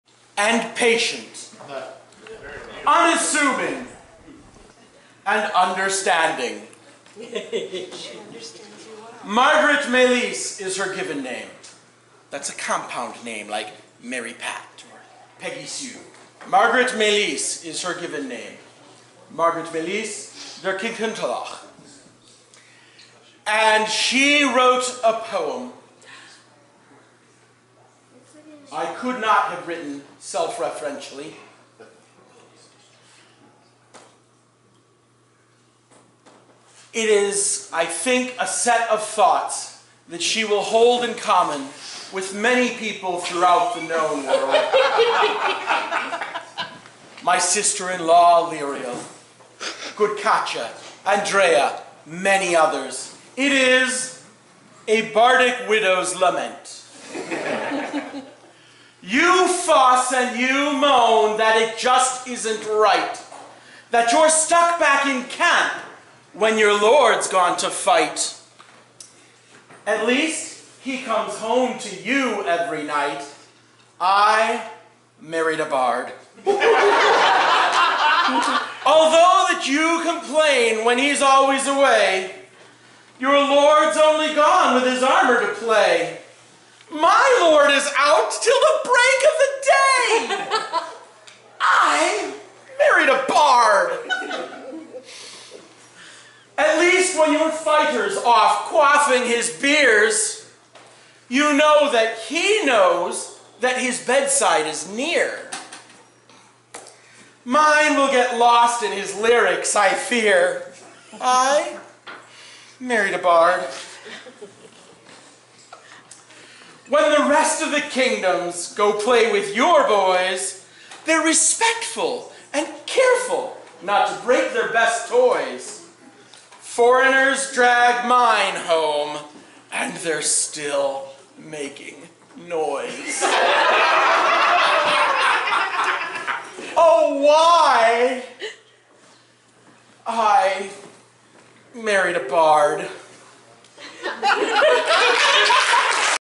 Blow Someone Else's Horn -- Dahrien
Recorded at Bardic Madness XXIII in the Kingdom of Northshield in the Shire of Falcon's Keep on April 27, 2013.  In the SCA we build upon the work of others and follow their inspiration to create new works of art. Perform the work of some other SCA person, either as tribute to someone who could not be with us at the event, or to show off the work of a friend who is there. Extra applause for memorizing or for performing something that is not well-known. We'll have a special chair up front for the author of your piece, if they are in attendance, to be recognized for their work! Originators: Ysolt/Dahrien Patron: Baroness Deirdre (HE Deirdre was played by HE Teffan.) The piece is poem by his lovely ladywife, Margaret Malise (Mysie).